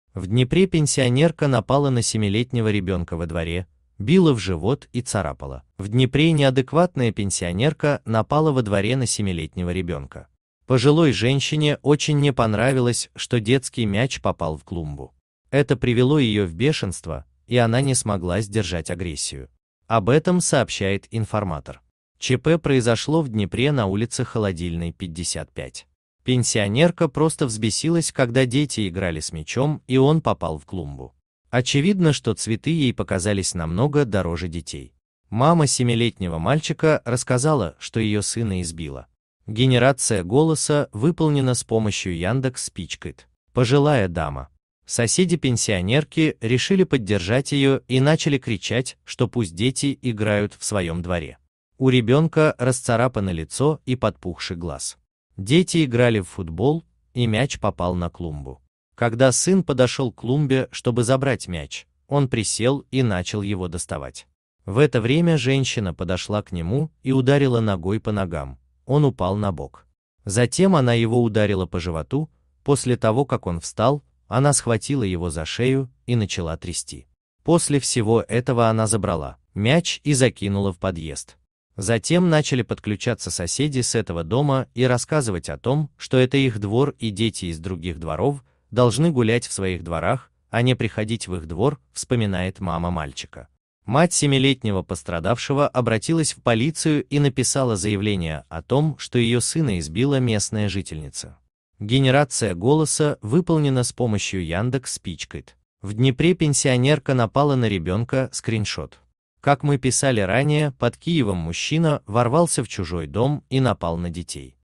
В Днепре пенсионерка напала на 7-летнего ребенка во дворе: била в живот и царапала.
Днепр В Днепре неадекватная пенсионерка напала во дворе на 7-летнего ребенка. Пожилой женщине очень не понравилось, что детский мяч попал в клумбу. Это привело ее в бешенство и она не смогла сдержать агрессию. Об этом сообщает "Информатор". ЧП произошло в Днепре на улице Холодильной, 55. Пенсионерка просто взбесилась, когда дети играли с мячом и он попал в клумбу. Очевидно, что цветы ей показались намного дороже детей. Мама 7-летнего мальчика рассказала, что ее сына избила пожилая дама. Соседи пенсионер